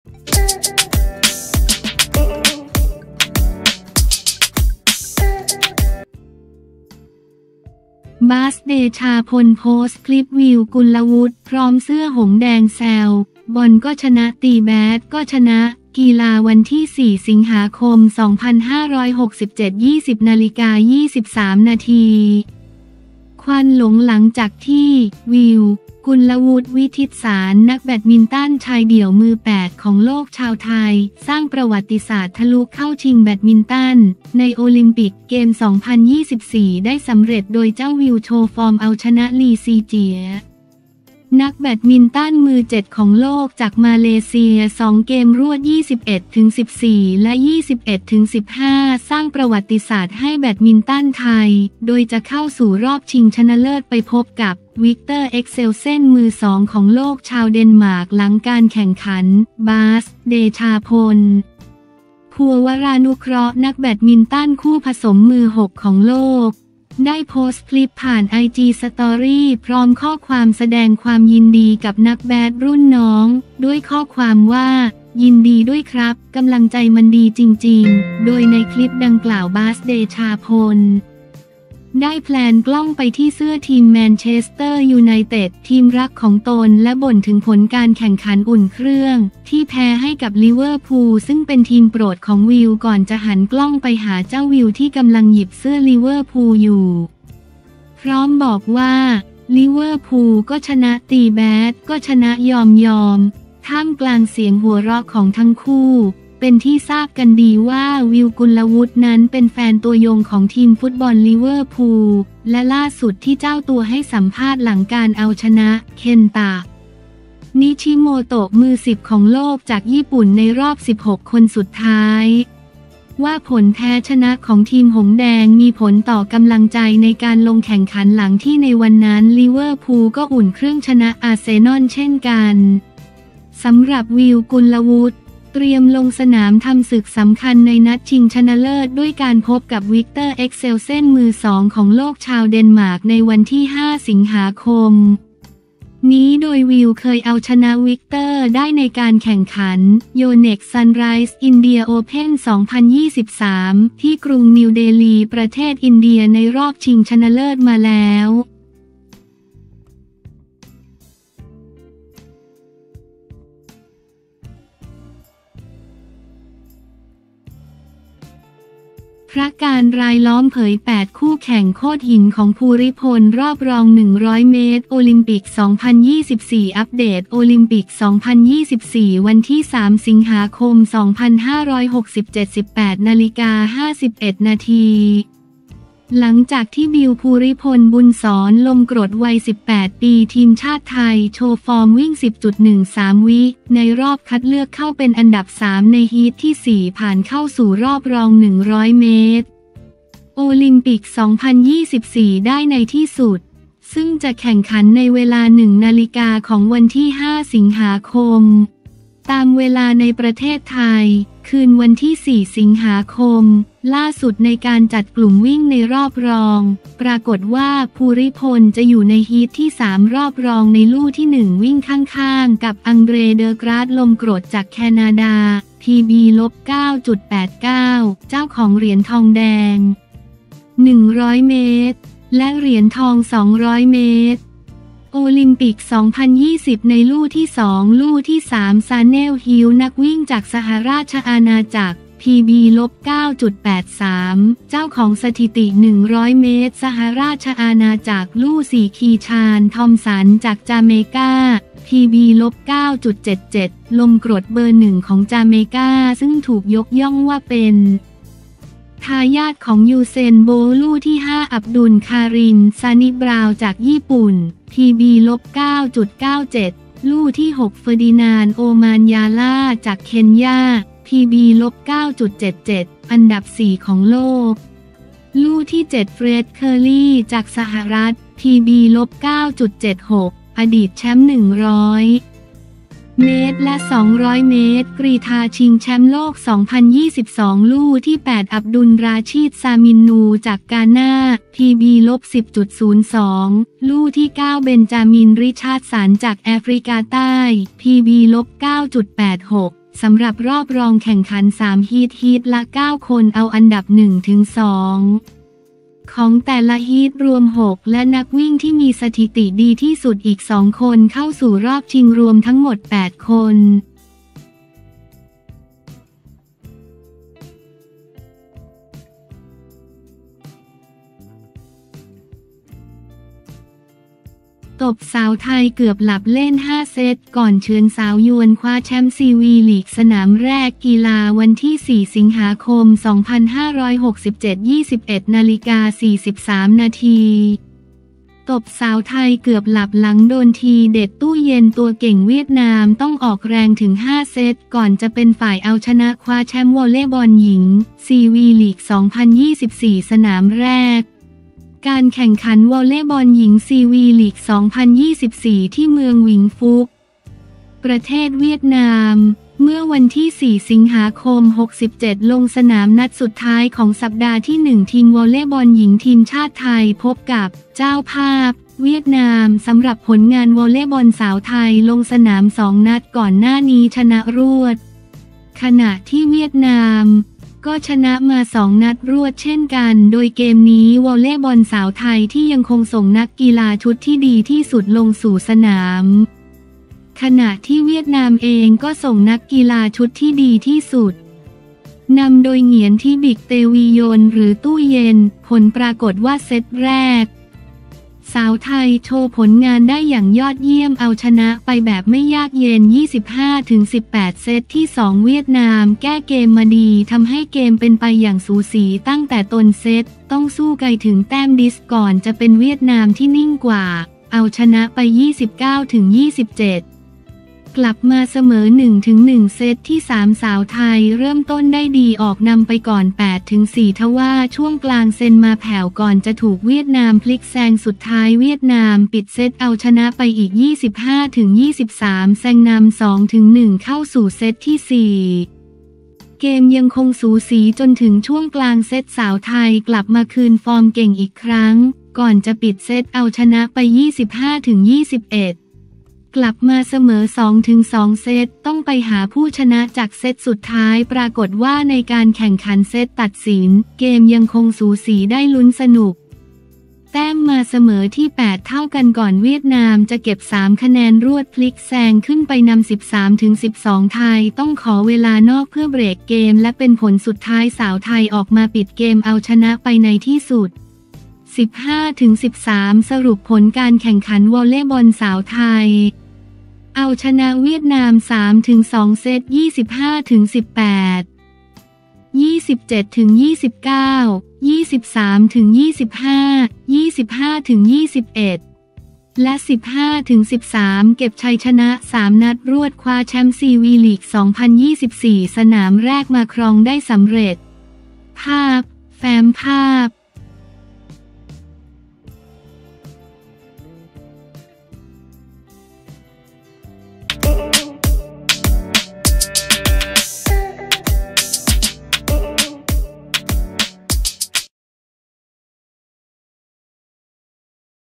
บาส เดชาพล โพสต์คลิป วิว กุลวุฒิ พร้อมเสื้อหงส์แดง แซว 'บอลก็ชนะ ตีแบดก็ชนะ'
กีฬา
4 ส.ค. 2567
20:23 น.
ควันหลงหลังจากที่ “วิว” กุลวุฒิ วิทิตศานต์ นักแบดมินตันชายเดี่ยว มือ 8 ของโลกชาวไทย สร้างประวัติศาสตร์ทะลุเข้าชิงแบดมินตัน ในโอลิมปิกเกมส์ 2024 ได้สำเร็จ

โดยเจ้าวิว โชว์ฟอร์มเอาชนะ ลี ซี เจี๋ย นักแบดมินตัน มือ 7 ของโลกจากมาเลเซีย 2 เกมรวด 21-14 และ 21-15 สร้างประวัติศาสตร์ให้แบดมินตันไทย โดยจะเข้าสู่รอบชิงชนะเลิศไปพบกับ วิกเตอร์ เอ็กเซลเซน มือ 2 ของโลกชาวเดนมาร์ก
หลังการแข่งขัน “บาส” เดชาพล พัววรานุเคราะห์ นักแบดมินตันคู่ผสม มือ 6 ของโลก ได้โพสต์คลิปผ่านไอจีสตอรี พร้อมข้อความแสดงความยินดีกับนักแบดรุ่นน้อง ด้วยข้อความว่า “ยินดีด้วยครับ กำลังใจมันดีจริงๆ”

โดยในคลิปดังกล่าว บาส เดชาพล ได้แพลนกล้องไปที่เสื้อทีม แมนเชสเตอร์ ยูไนเต็ด ทีมรักของตน และบ่นถึงผลการแข่งขันอุ่นเครื่อง ที่แพ้ให้กับ ลิเวอร์พูล ซึ่งเป็นทีมโปรดของวิว ก่อนจะหันกล้องไปหาเจ้าวิว ที่กำลังหยิบเสื้อลิเวอร์พูลอยู่ พร้อมบอกว่า “ลิเวอร์พูลก็ชนะ ตีแบดก็ชนะ ยอมๆ” ท่ามกลางเสียงหัวเราะของทั้งคู่
เป็นที่ทราบกันดีว่า วิว กุลวุฒิ นั้นเป็นแฟนตัวยงของทีมฟุตบอลลิเวอร์พูล และล่าสุดที่เจ้าตัวให้สัมภาษณ์หลังการเอาชนะ เคนตะ นิชิโมโตะ มือ 10 ของโลกจากญี่ปุ่นในรอบ 16 คนสุดท้าย ว่าผลแพ้ชนะของทีมหงส์แดง มีผลต่อกำลังใจในการลงแข่งขัน หลังที่ในวันนั้น ลิเวอร์พูล ก็อุ่นเครื่องชนะ อาร์เซนอล เช่นกัน

สำหรับ วิว กุลวุฒิ เตรียมลงสนามทำศึกสำคัญ ในนัดชิงชนะเลิศ ด้วยการพบกับ วิกเตอร์ เอ็กเซลเซน มือ 2 ของโลกชาวเดนมาร์ก ในวันที่ 5 ส.ค. นี้ โดย วิว เคยเอาชนะ วิกเตอร์ ได้ในการแข่งขัน “โยเน็กซ์ ซันไรส์ อินเดีย โอเพ่น 2023” ที่กรุงนิวเดลี ประเทศอินเดีย ในรอบชิงชนะเลิศมาแล้ว